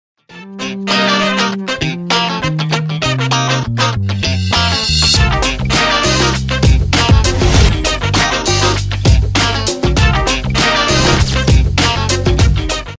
¿Limits or NoLimits? Conoce el final y gana un Honor 7X
El 5 de Diciembre presentamos nuestro nuevo Honor7X.

Prepárate para adentrarte en un mundo Sin Límites con su pantalla infinita.